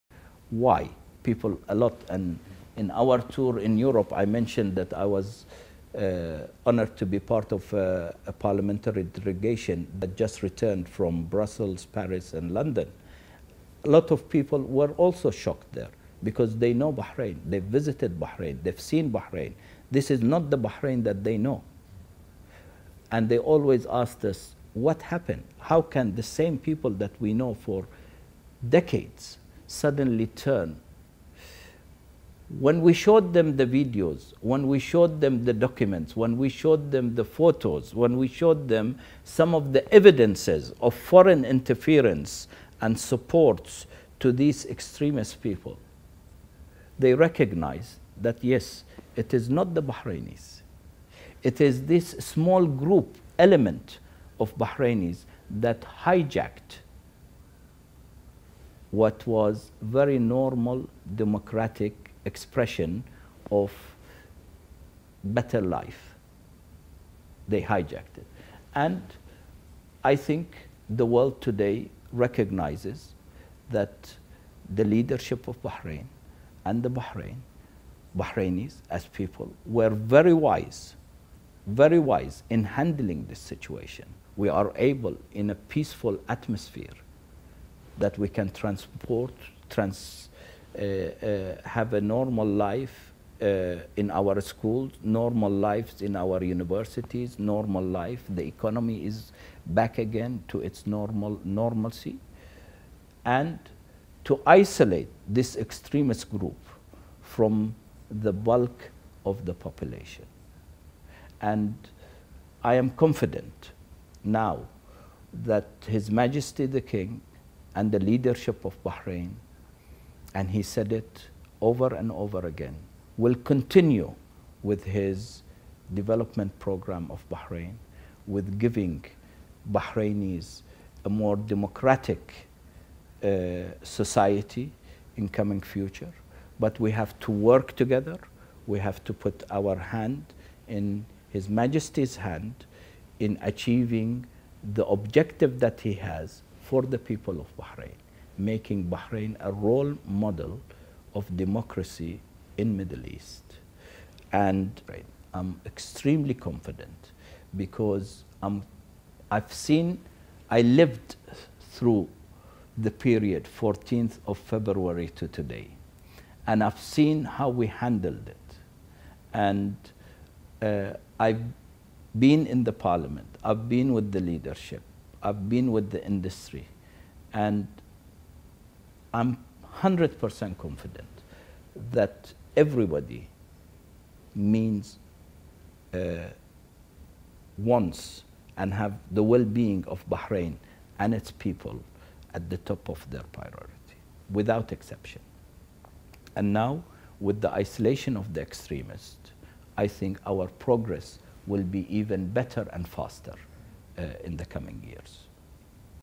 Why Were There Protests and Riots in Bahrain?
Marcopolis.net Video Interview with Abdulrahman A.Hussain Jawahery, President of Gulf Petrochemical Industries Co. (GPIC) and Member of Parliament.

Mr. Jawahery explains why there were riots and protest in Bahrain. The protests turned violent after the peaceful movement has been hijacked by the extremists. Why this happened?

To read the full transcript of the MarcoPolis interview with Mr. Jawahery of Gulf Petrochemical Industries Co. (GPIC) and Member of Parliament visit Marcopolis.net webpage